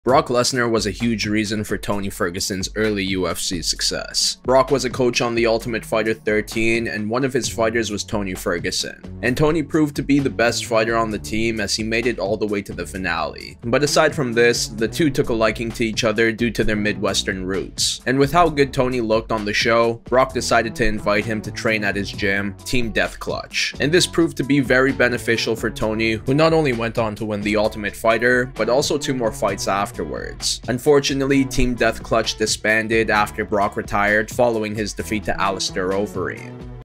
Brock Lesnar was a huge reason for Tony Ferguson’s early UFC success
Kiiyon Kimura Podcast